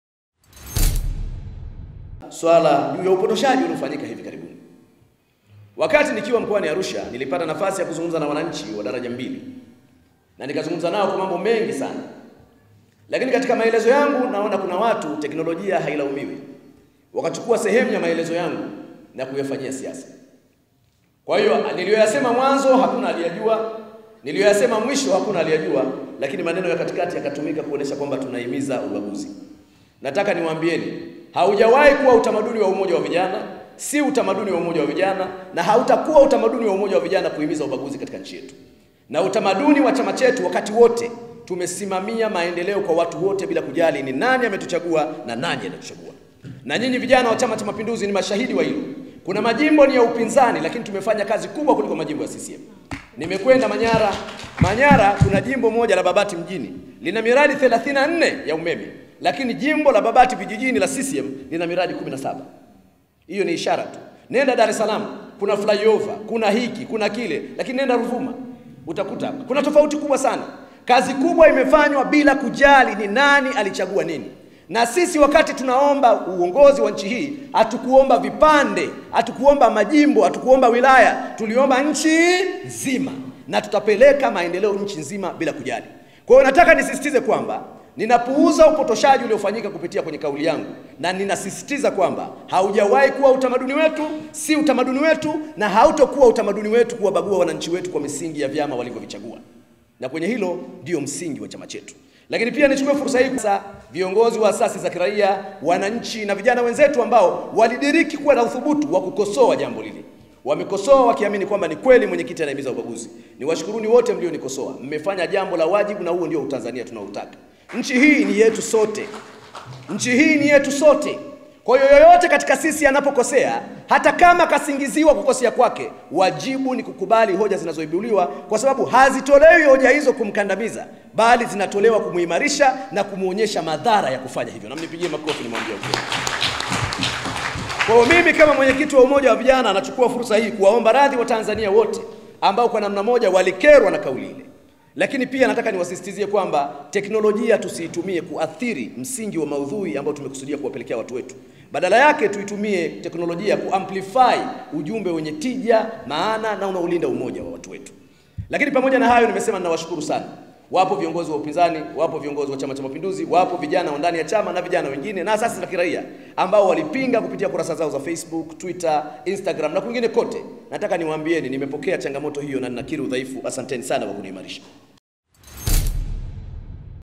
M/KITI UVCCM AOMBA WATANZANIA MSAMAHA "NAKIRI UDHAIFU"
Mwenyekiti wa Umoja wa Vijana Chama cha mapinduzi (UVCCM) Kheri James ametolea ufafanuzi video clip yake iliyosambaa akionekana kutoa maneno ya Ubaguzi kwa maeneo ambayo hawajaichagua CCM.